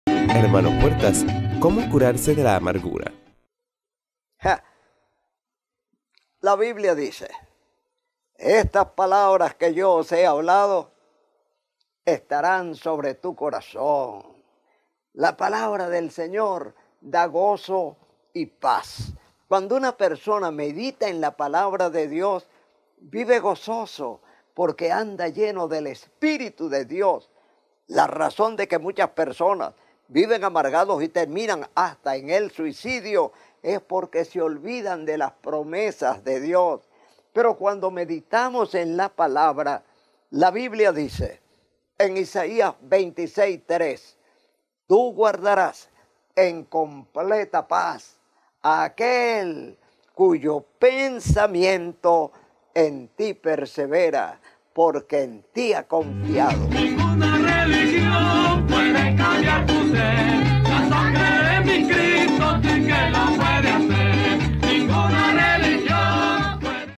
hnopuertasresponde #383 ¿Como curarme de la amargura?
pastor Jaime Puertas:
¿Cómo curarse de la amargura?